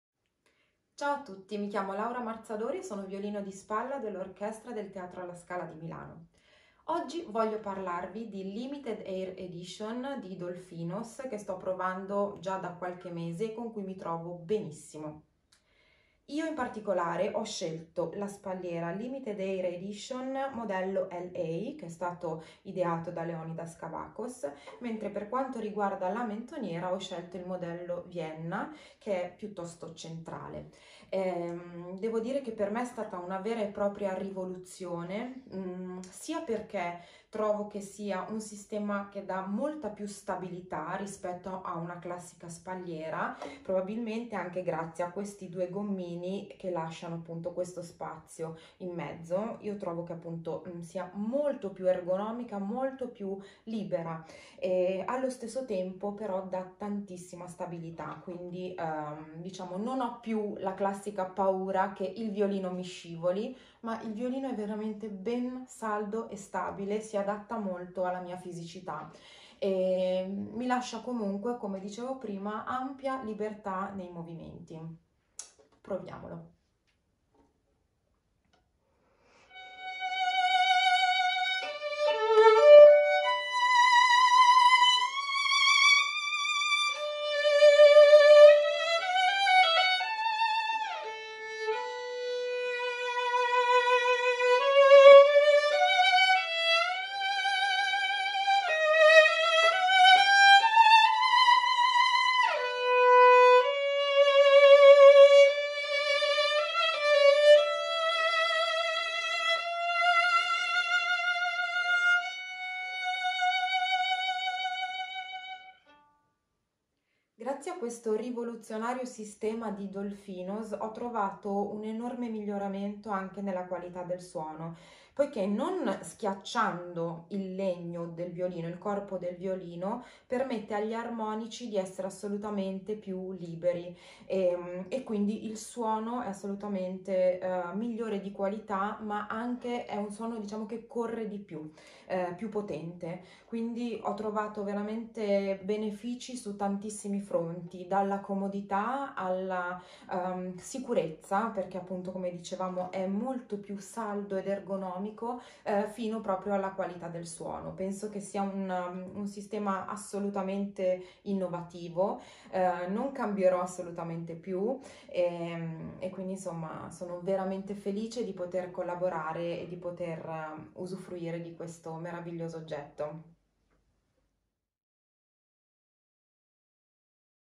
Laura Marzadori plays on the DOLFINOS System
At the age of just 25 years old, the commission members with Daniel Barenboim as its president, unanimously elected Laura Marzadori as concertmistress at the Scala Theater Orchestra in Milan. Watch Laura Marzadori explain the benefits she notices with the DOLFINOS System. She uses the L.A. edition with a Vienna chinrest.

The DOLFINOS Rest System is available for violin and viola.